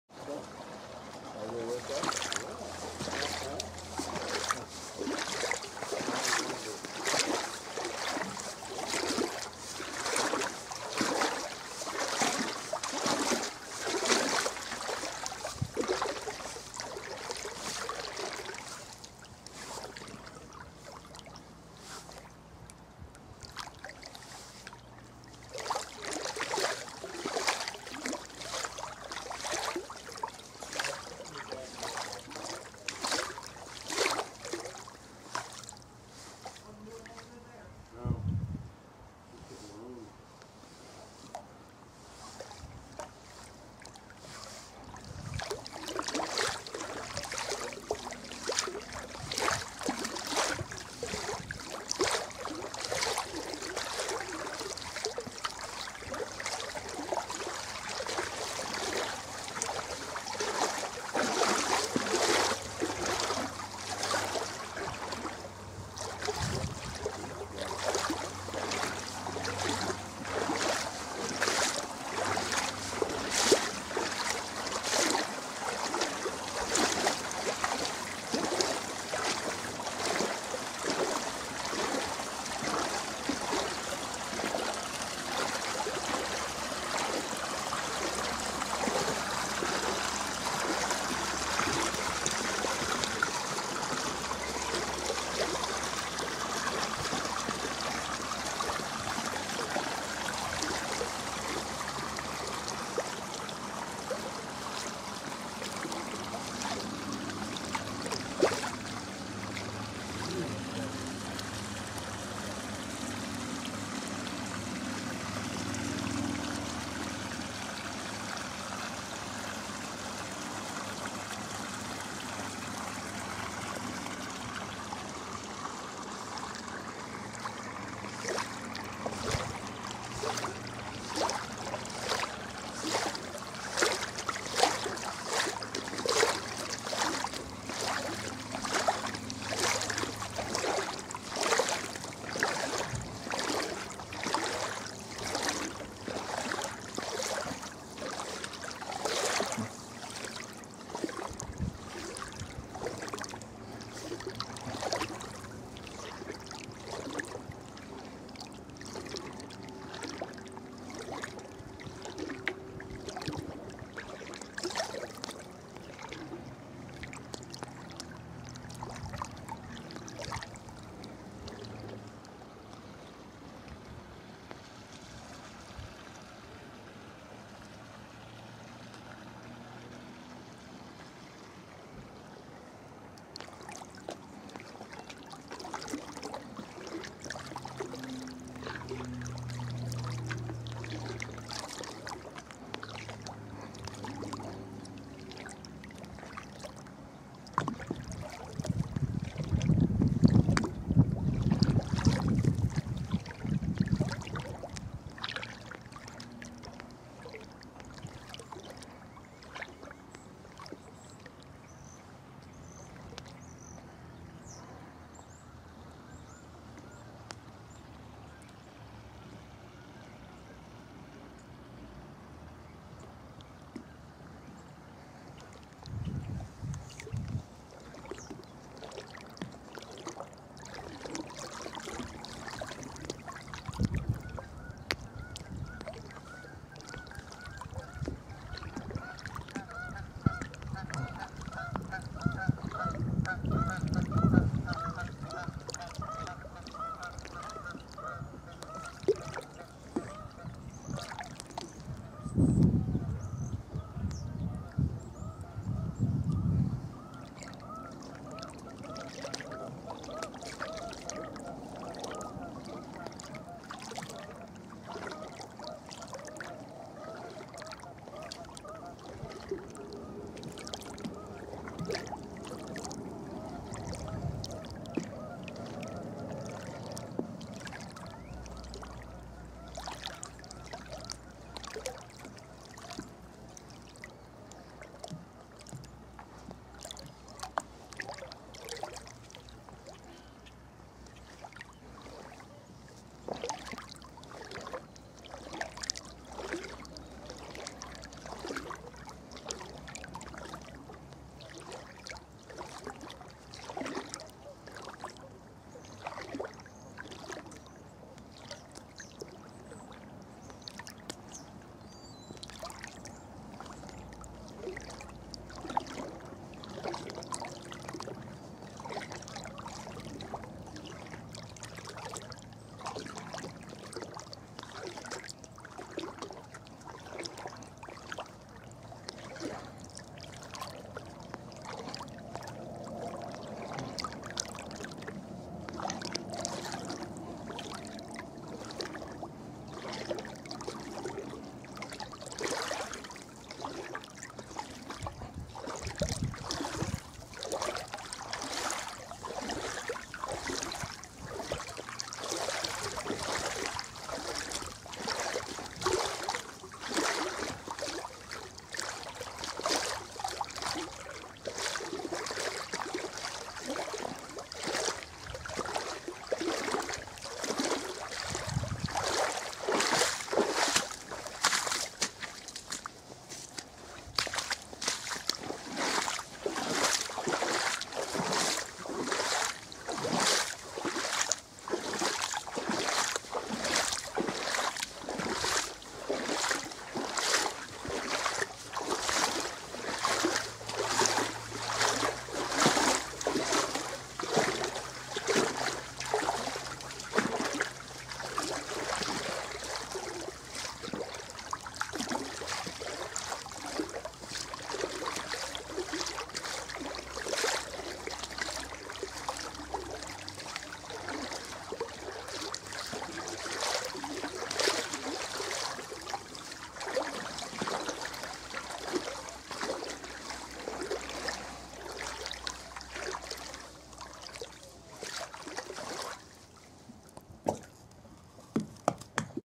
Channel Lake Flooding - A Wading Tour
Here is an up close and personal look at the flooding in our neck of the woods.  We've been to this rodeo before (just not to this extreme) so the power has been shut off in the affected area.  I always wear insulated waders when working in flood waters & I would recommend you do the same.  All the debris piles consist of stuff we have pulled out of the water over the past few days.  Fortunately, these RVs are used as weekend get-aways so nobody lives in them full time.  There are many homeowners in the area that are not so lucky.